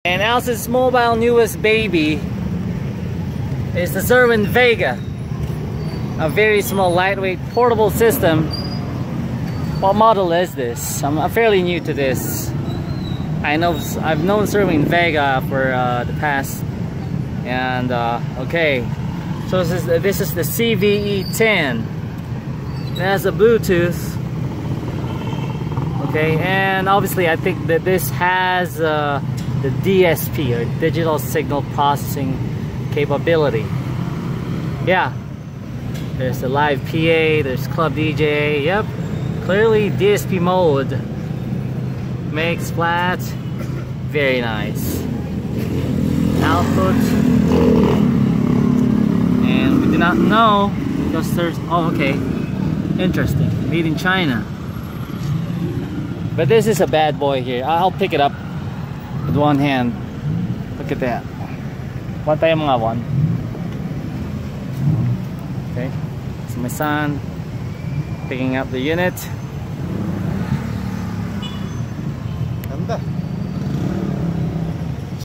Cerwin Vega CVE-10 quick look
back to DJ mode. Just a quick look at this little portable speaker is lightweight, tough, powerful, with DSP and bluetooth.